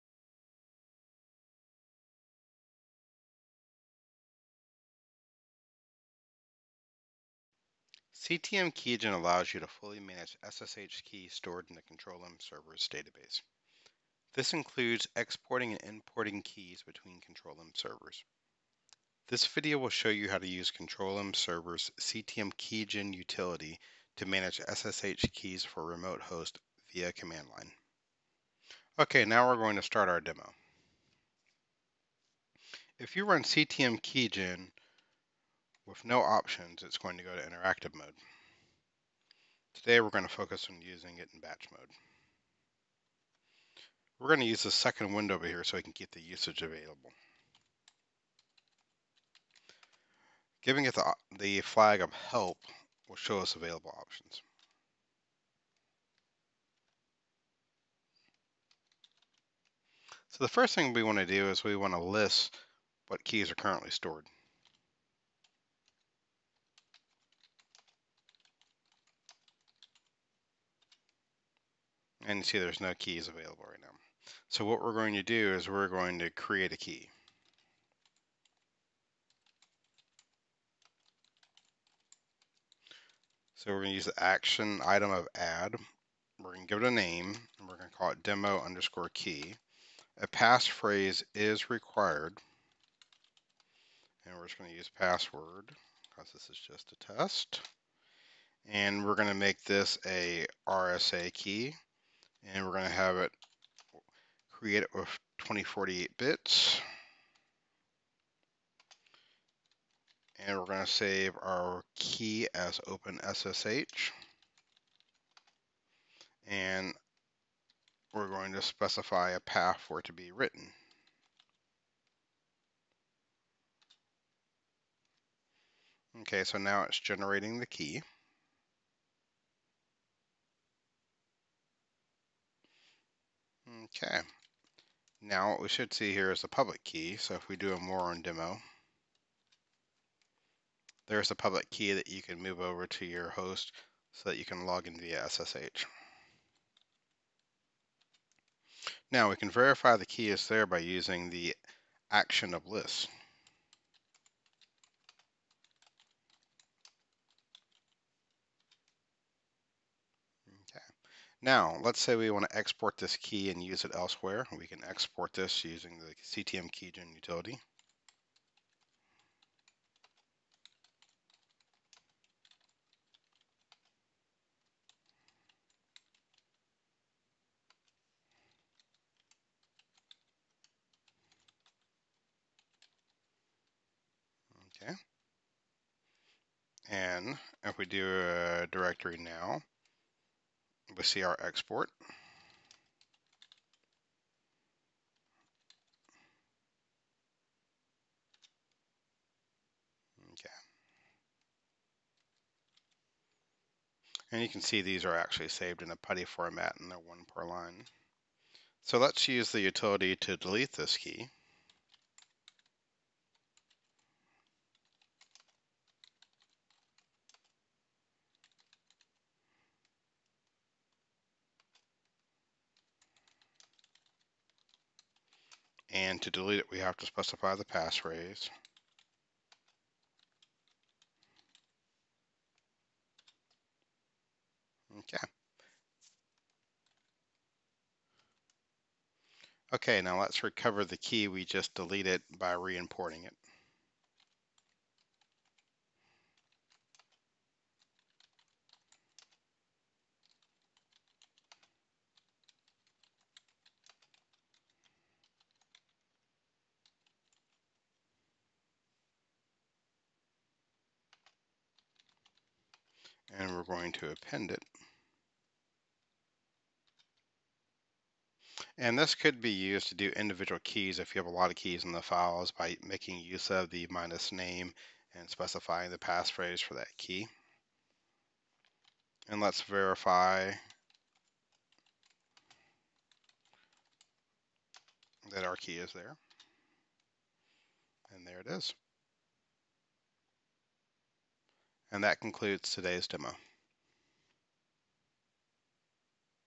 How to use ctmkeygen to manage SSH keys via the command line
This video solution demonstrates how to manage SSH keys via the command line using ctmkeygen.